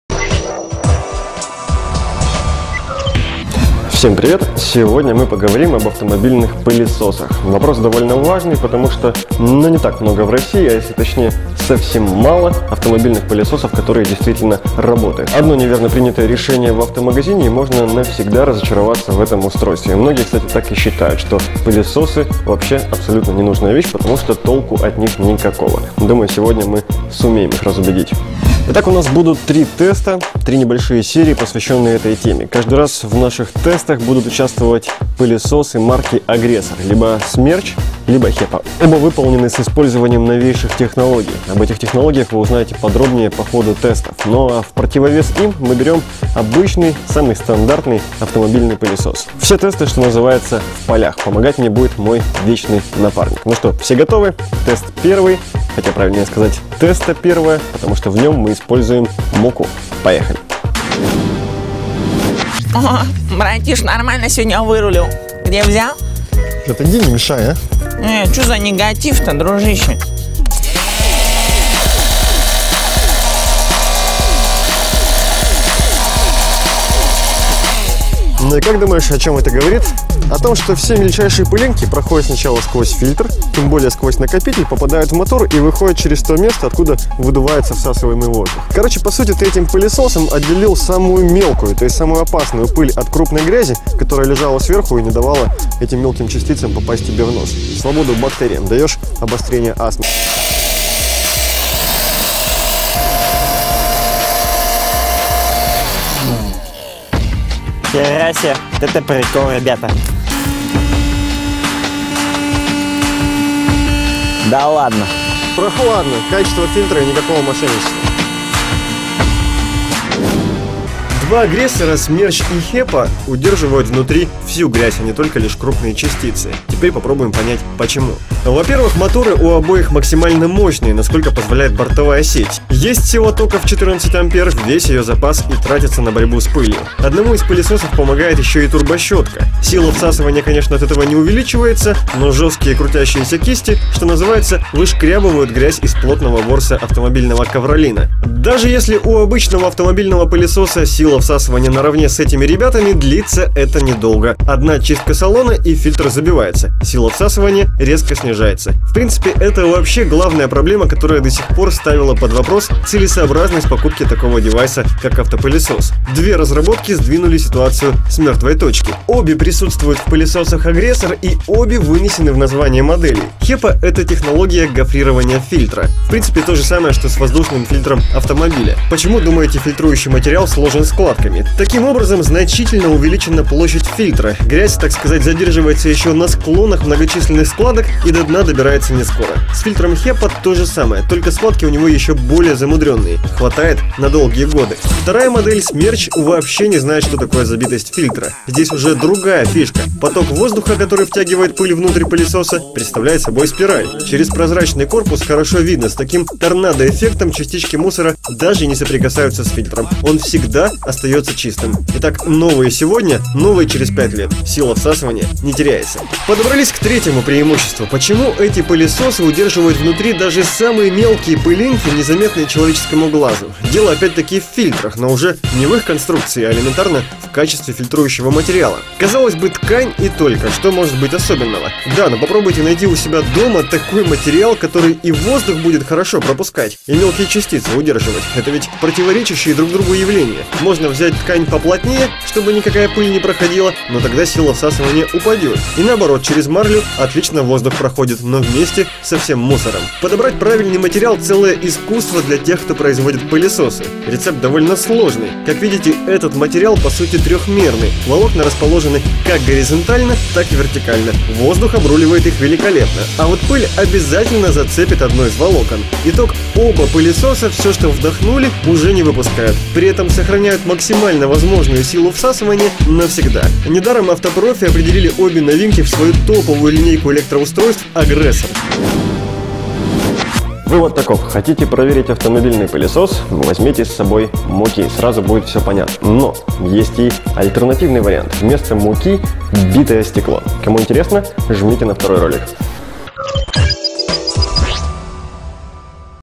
Автопылесос тестируют мукой — первый тест пылесосов "Агрессор"
Первый тест автопылесоса "Агрессор". Задача — собрать муку. Проверяем HEPA фильтр и турбощетку в автомобильных пылесосах от ТД "Автопрофи". Смотрим, что получилось!